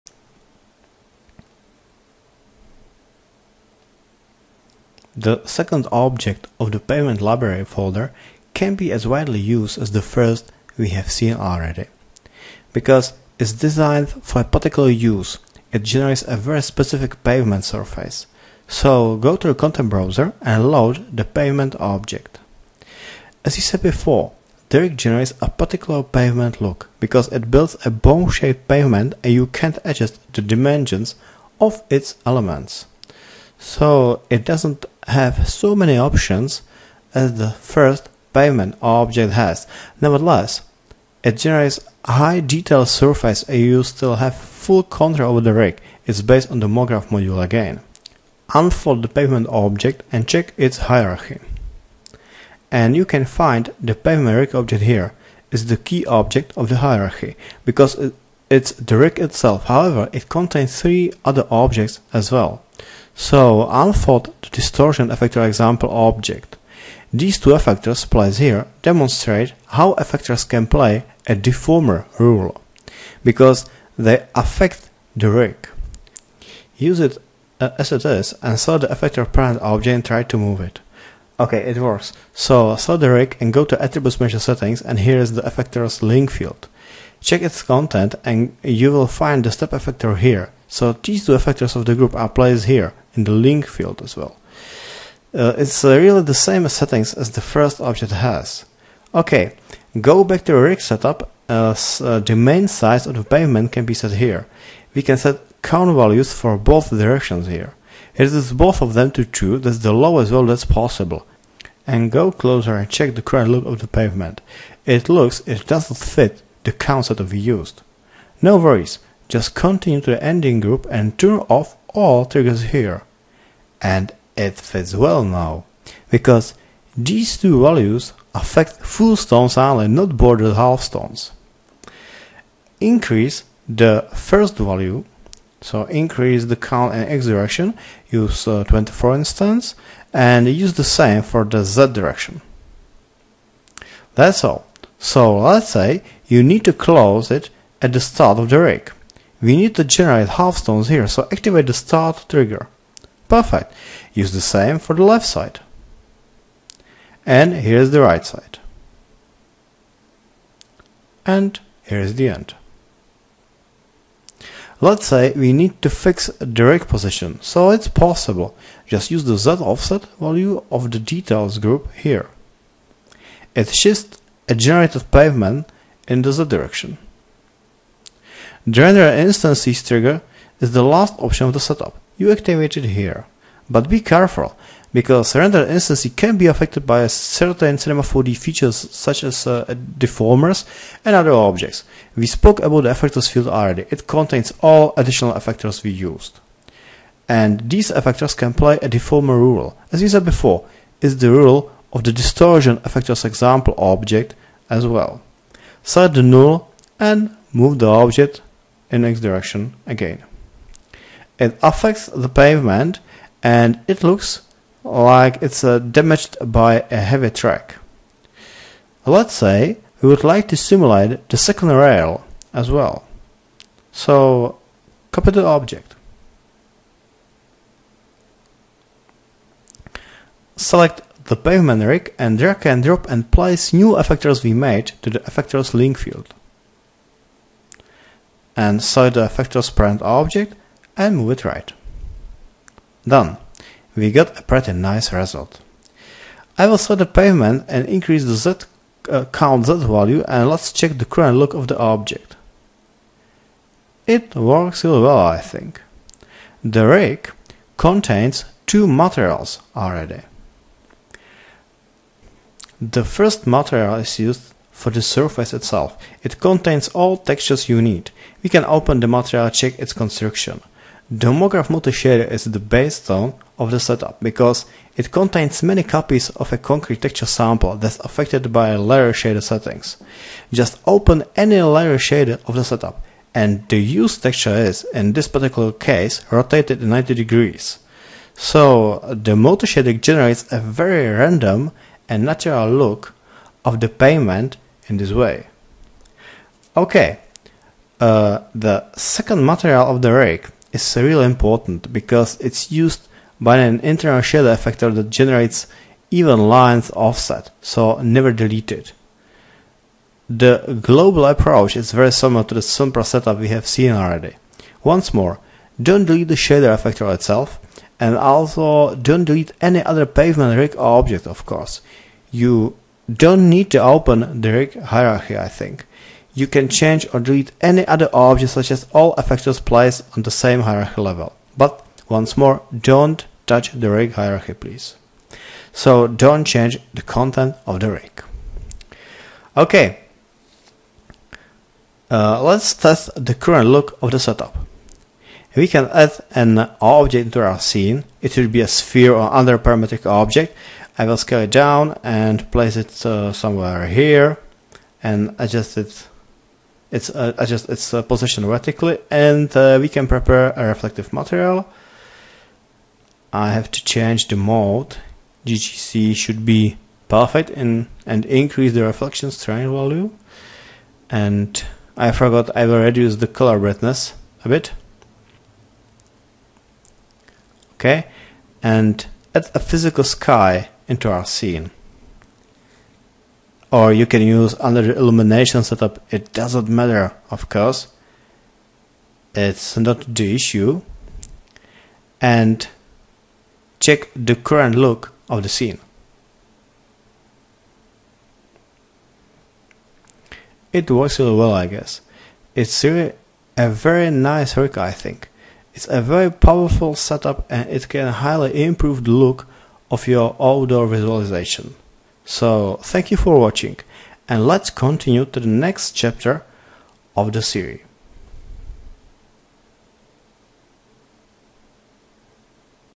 CINEMA 4D R16 Studio Content browser rig tutorial, Bone pavement rig (04)
The Pavement modular library contains two different setups. The first is a very complex and well textured Bone looking pavement rig. You can produce a very realistic pavement with all the disturbances and details you need.

Rig designed by Pavel Zoch
Tutorial mentor: Pavel Zoch